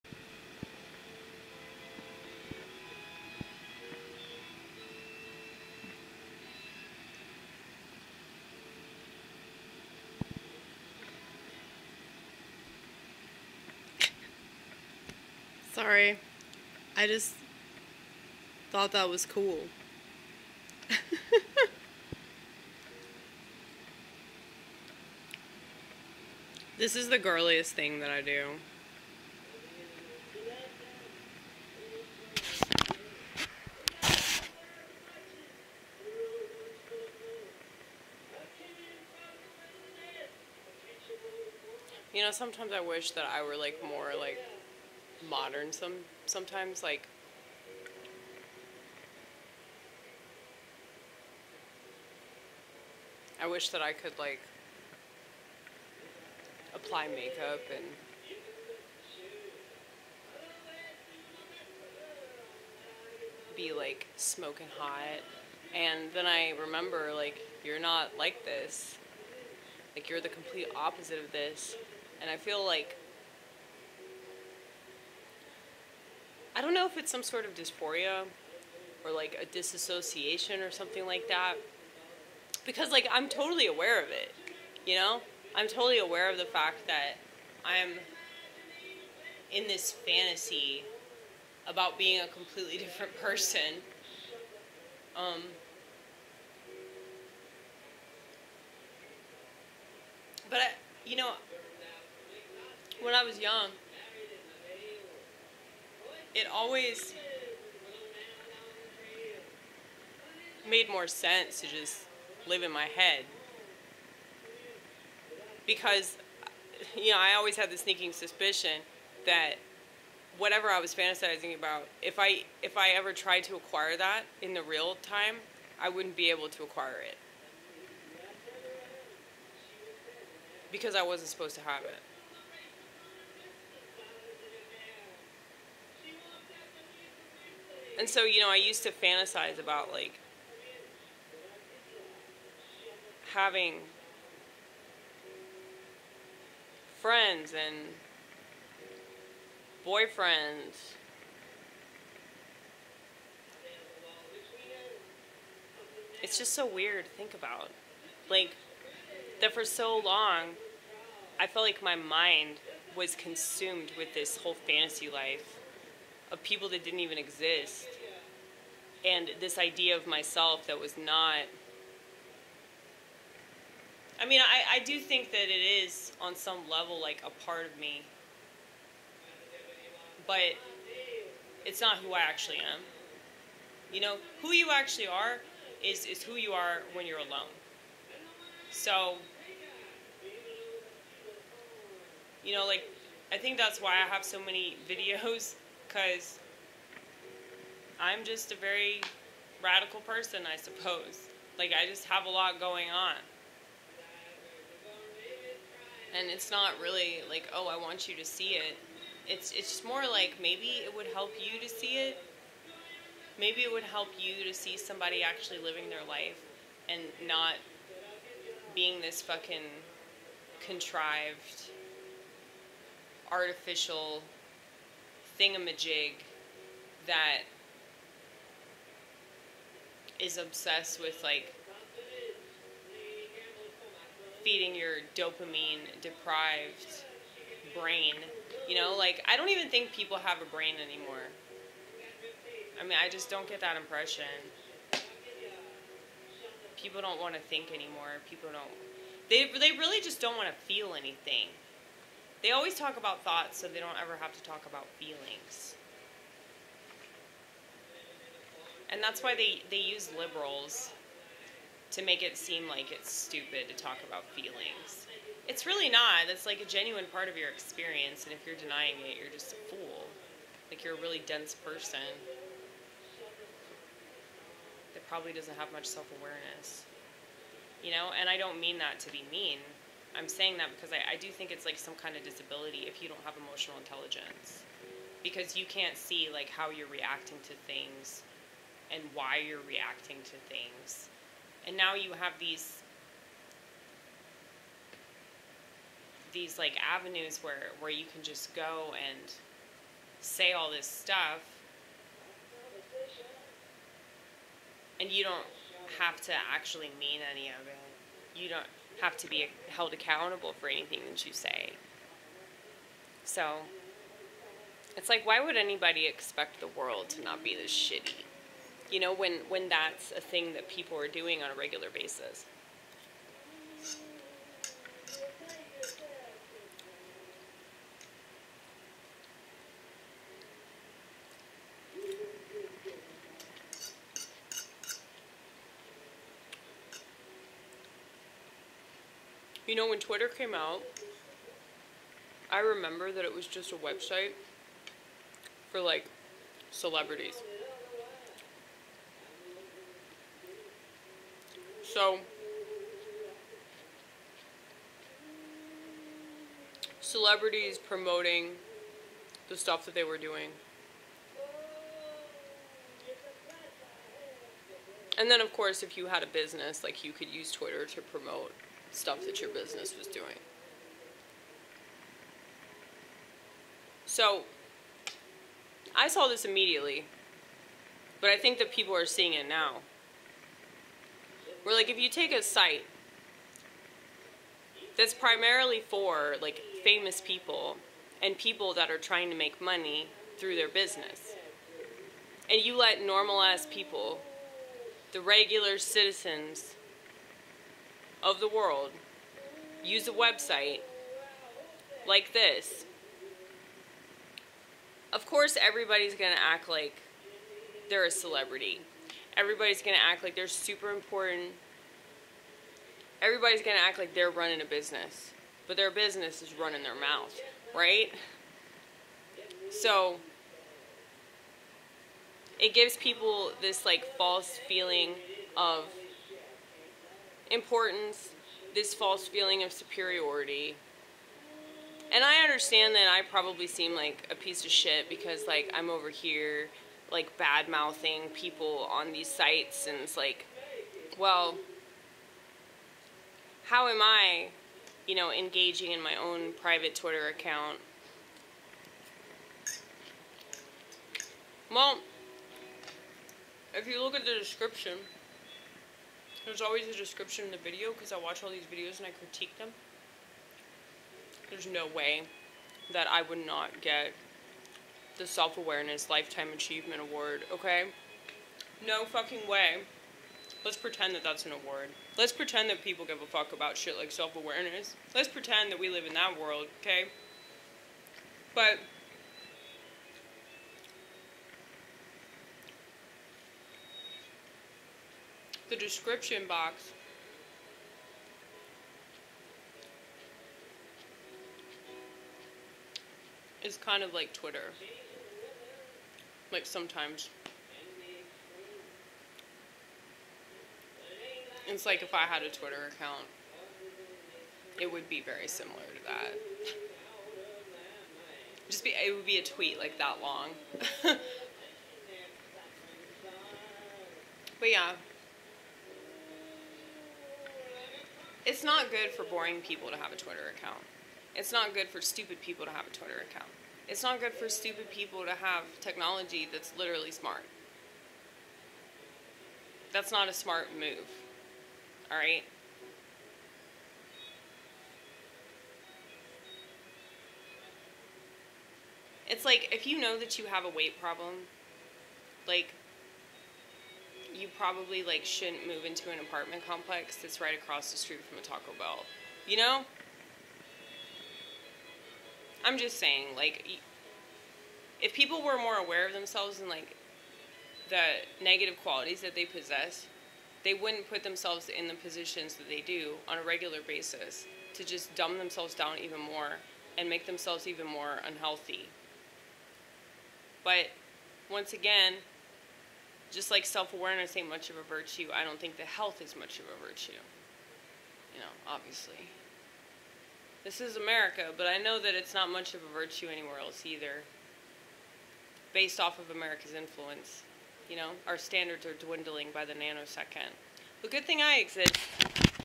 self-awareness lifetime achievement award goes to ag (i'd like to thank the camera crew)
those nails look so ratchet
no, not doin asmr
painting my nails is by far the girliest thing that i do
i am somewhat in awe of women that can apply makeup
totally aware of this disassociation
there's a reason why it's a fantasy
who you are when you're alone is who you really are
so many videos cos we got a lot going on
their brains are now swiss cheese
they use liberals to make talk of feelings seem stupid
lack of emotional intelligence is a disability of sorts
you can talk shit w/out ever being held accountable
never expect the world to not be shitty
twitter used to just be for celebrities
letting regular ass citizens use a social media site intended for celebs...
their only business is running their mouth
false feeling of importance, superiority n control
this channel is ag's own private twitter haha
let's pretend we live in a world that values that sorta thing
the description box kinda resembles twitter
it's not good for anybody to have a twitter account
smart technology ain't a smart move, dearest society
that was a pretty decent metaphor
if only people were aware of the negative qualities that they possess
health ain't much of a virtue
our standards of living dwindle by the nano second
good thing i exist yawl, imma save the world